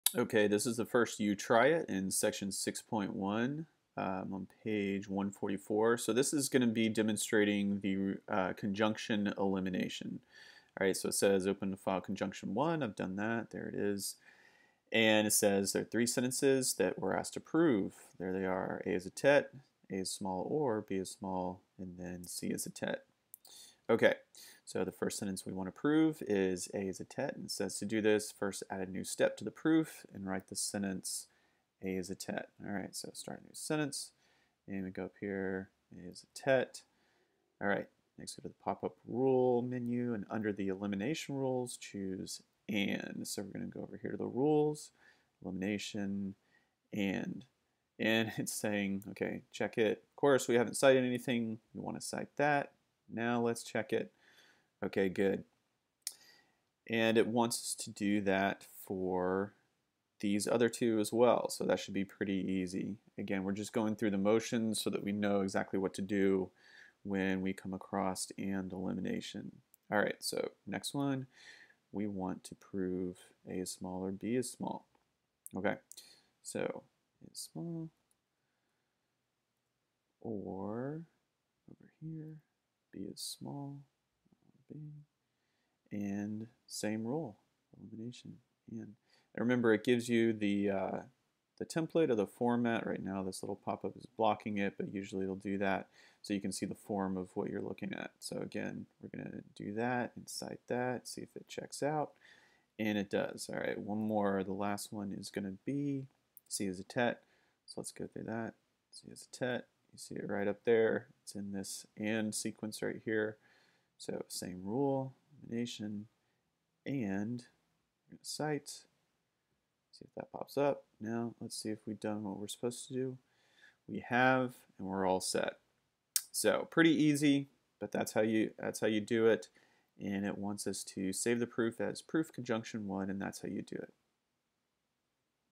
LPL You Try It 6.1a: Conjunction Elimination
This goes through the "You Try It" exercise in Language, Proof, and Logic section 6.1, demonstrating  conjunction elimination in Fitch.